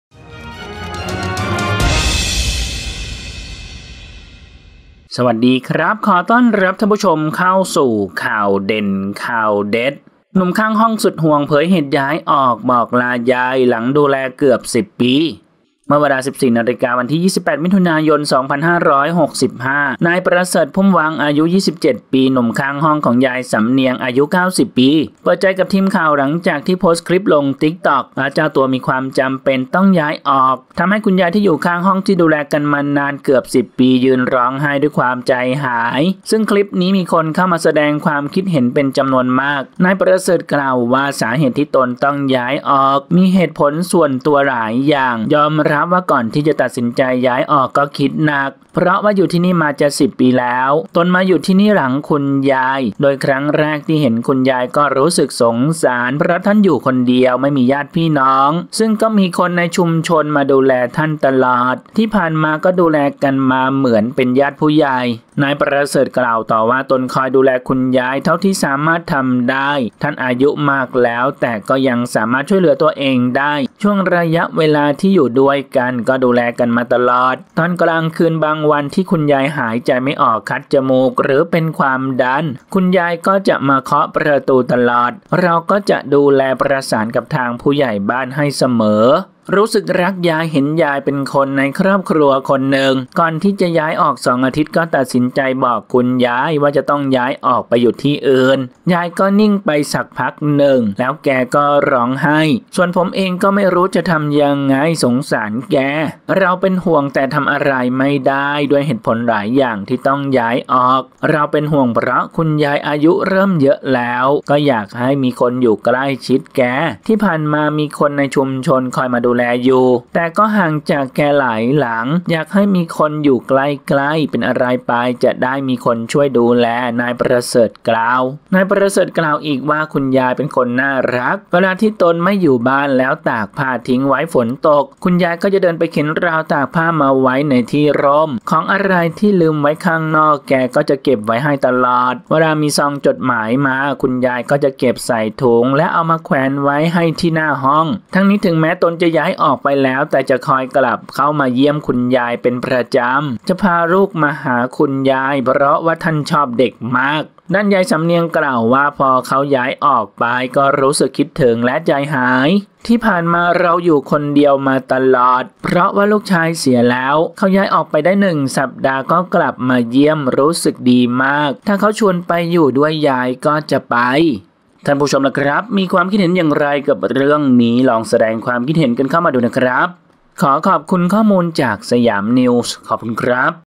หนุ่มข้างห้อง สุดห่วง เผย เหตุย้ายออกบอกลายาย หลังดูแลเกือบ 10 ปี
เมื่อเวลา 14.00 น. วันที่ 28 มิ.ย.2565 นายประเสริฐ พุ่มวัง อายุ 27 ปี หนุ่มข้างห้องของ ยายสำเนียง อายุ 90 ปี เปิดใจกับ ทีมข่าว หลังจากที่โพสต์คลิปลง Tik Tok ว่าเจ้าตัวมีความจำเป็นต้องย้ายออก ทำให้คุณยายที่อยู่ข้างห้องที่ดูแลกันมานานเกือบ 10 ปี ยืนร้องไห้ด้วยความใจหาย ซึ่งคลิปนี้มีคนเข้ามาแสดงความคิดเห็นเป็นจำนวนมาก

นายประเสริฐ กล่าวว่า สาเหตุที่ตนต้องย้ายออกมีเหตุผลส่วนตัวหลายอย่าง ยอมรับว่าก่อนที่จะตัดสินใจย้ายออกก็คิดหนักเพราะว่าอยู่ที่นี่มาจะ 10 ปีแล้ว ตนมาอยู่ที่นี่หลังคุณยาย โดยครั้งแรกที่เห็นคุณยายก็รู้สึกสงสารเพราะท่านอยู่คนเดียว ไม่มีญาติพี่น้อง ซึ่งก็มีคนในชุมชนมาดูแลท่านตลอด ที่ผ่านมาก็ดูแลกันมาเหมือนเป็นญาติผู้ใหญ่

นายประเสริฐ กล่าวต่อว่า ตนคอยดูแลคุณยายเท่าที่สามารถทำได้ ท่านอายุมากแล้ว แต่ก็ยังสามารถช่วยเหลือตัวเองได้ ช่วงระยะเวลาที่อยู่ด้วยกันก็ดูแลกันมาตลอด ตกกลางคืนบางวันที่คุณยายหายใจไม่ออก คัดจมูก หรือเป็นความดัน คุณยายก็จะมาเคาะประตูตลอด เราก็จะดูแล ประสานกับทางผู้ใหญ่บ้านให้เสมอ